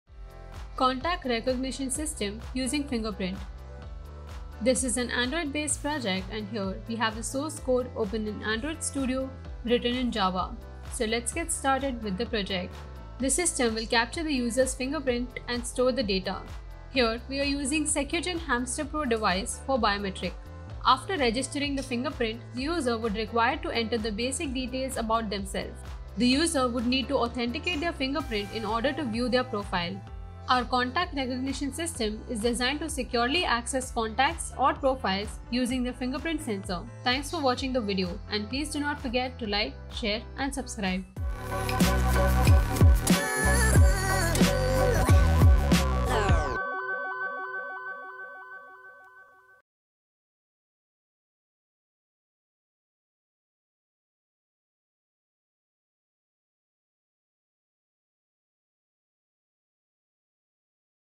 App For Contact Recognition using Fingerprint | Android Project Ideas
We have introduced our Contact Recognition System using Fingerprint Sensor to tackle the problem of memorizing passwords and maintaining the security of different identities. A fingerprint is a graphical pattern of ridges and valleys on the surface of a human finger. Due to the uniqueness and permanence of fingerprints, they are among the most reliable human characteristics that can be used for personal identification.